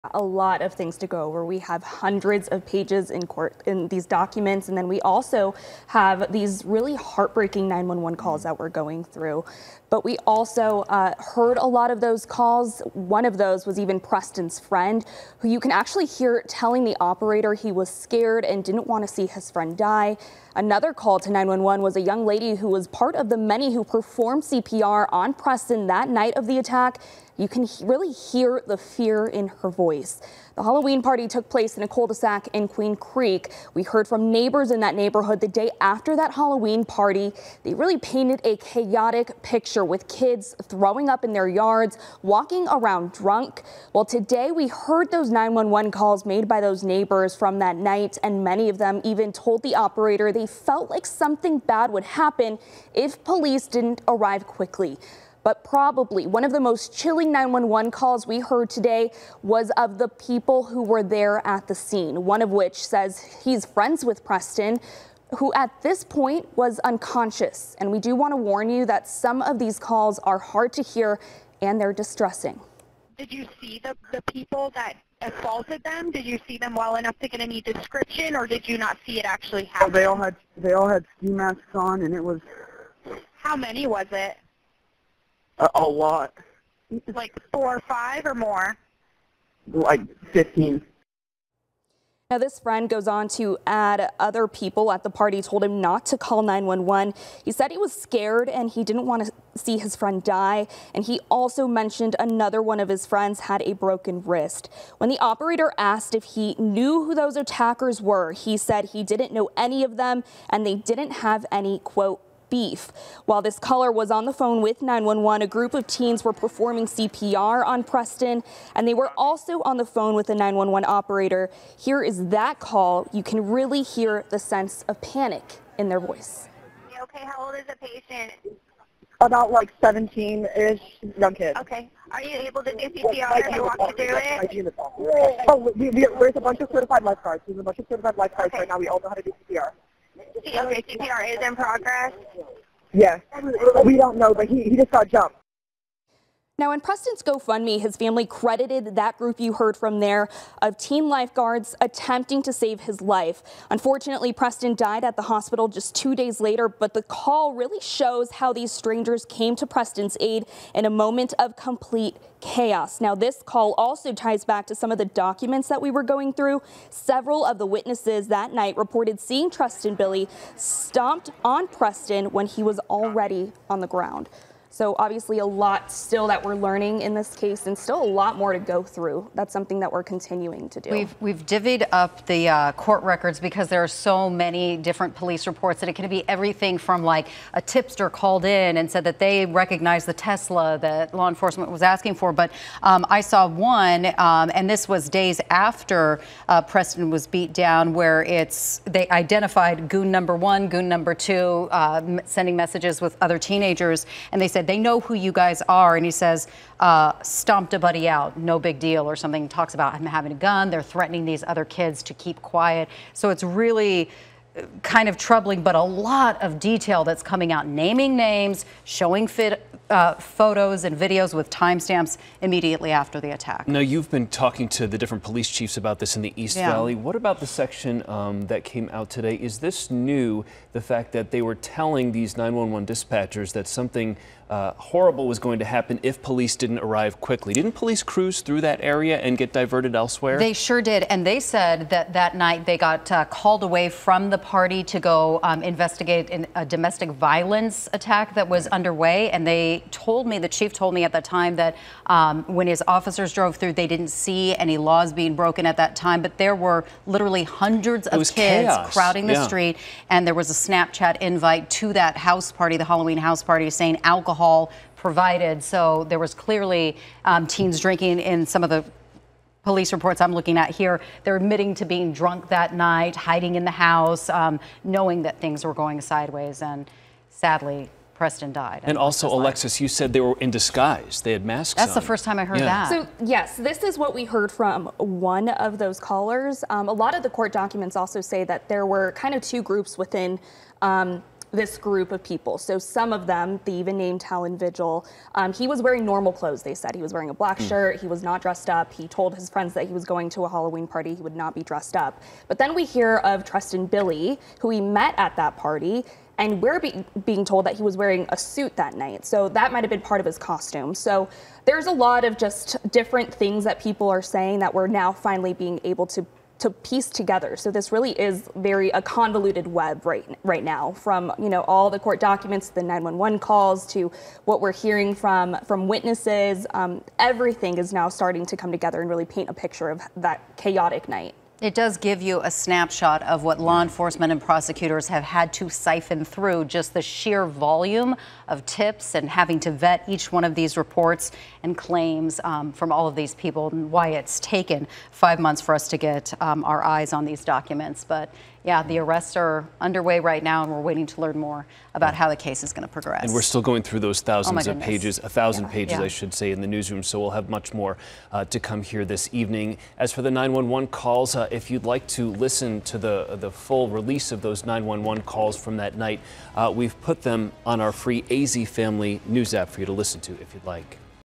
911 calls, police report released in Preston Lord murder case
Queen Creek police released a nearly 1,100-page report that details interviews with witnesses and suspects, case leads and more.